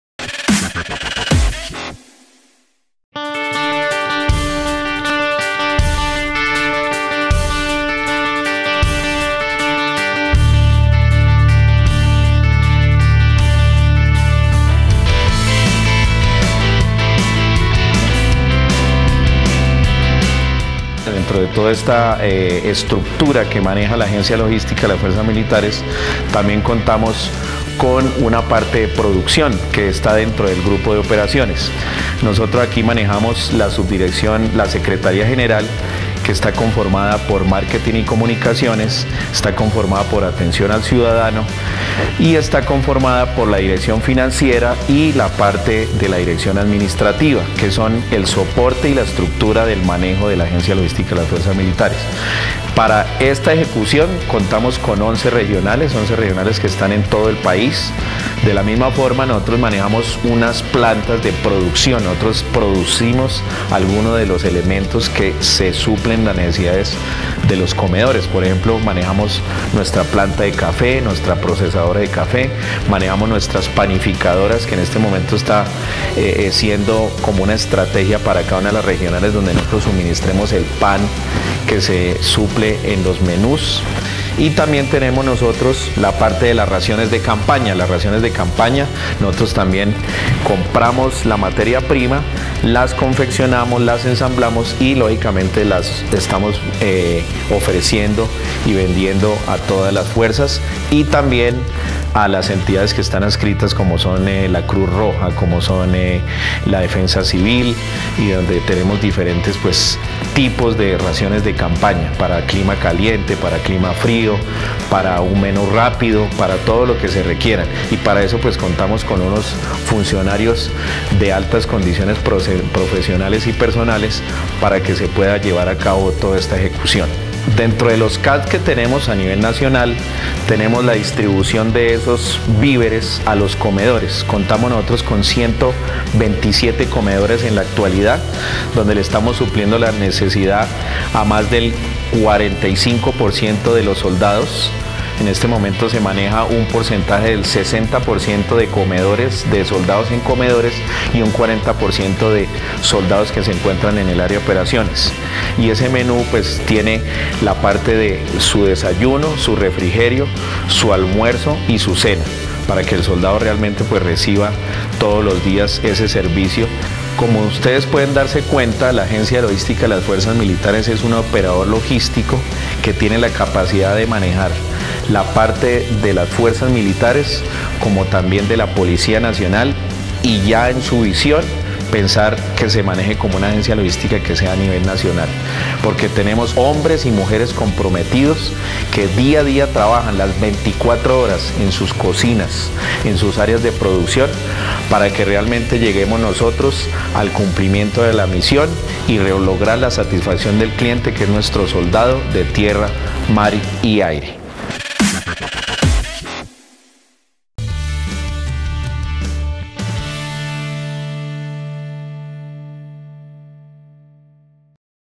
InsideGSED Agencia Logística, capitulo 2
Conozca los detalles del funcionamiento de la Agencia Logística, una de las empresas del GSED. Este es el segundo capitulo de la MiniSerie